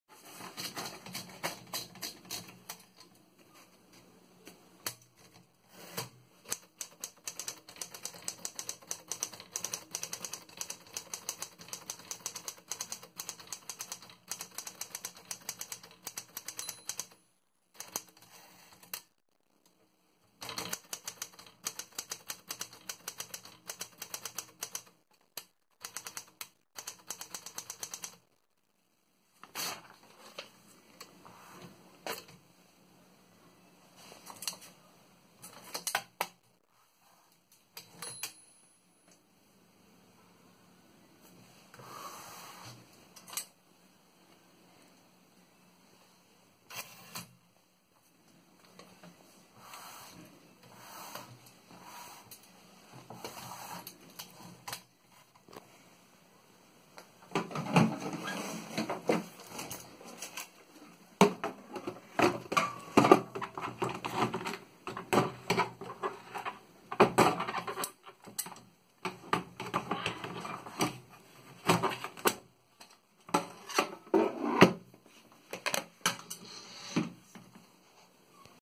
LC SMITH & CORONA STANDARD typewriter 1936
maquina de escribir antigua, portatil, marca LC & SMITH modelo CORONA STANDARD, del año 1936 (por su num. de serie), con su estuche original de madera.